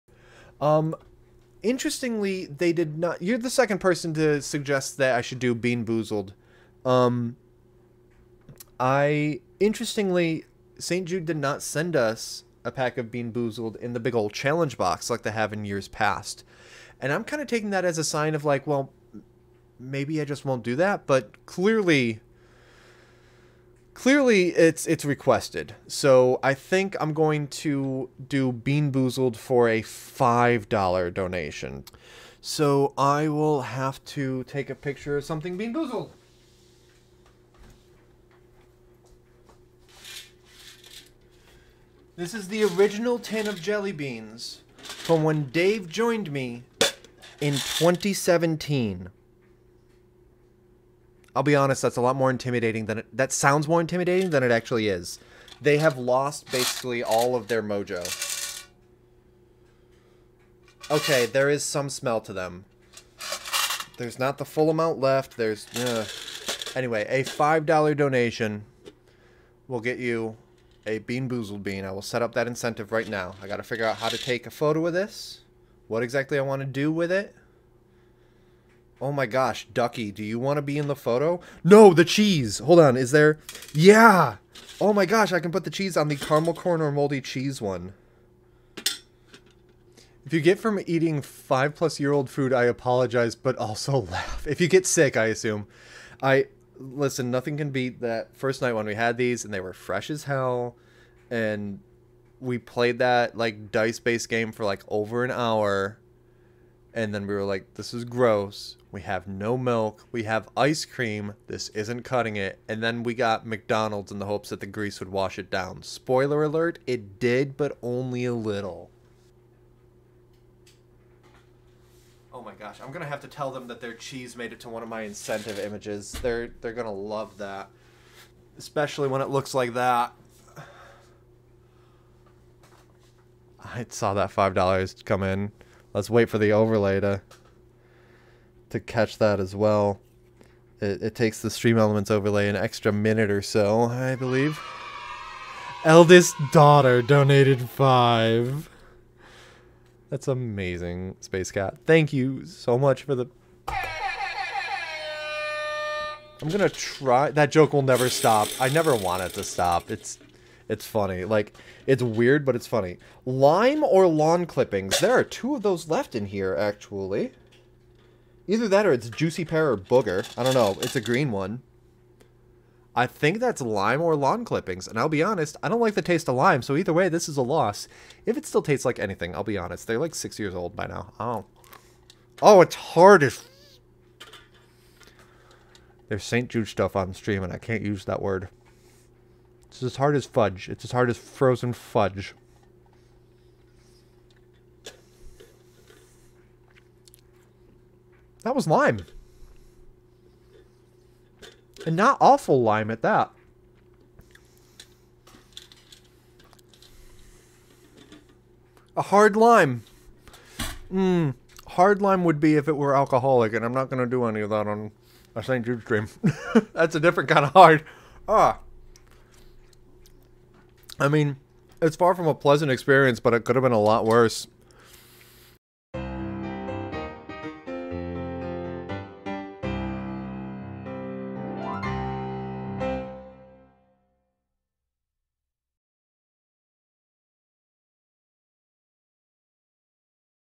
Bean Boozled is Back - Just Chatting (Stream Highlight)
Back due to popular request, my taste buds and probably my nose as well hates you (but it's for the kids, so they can deal).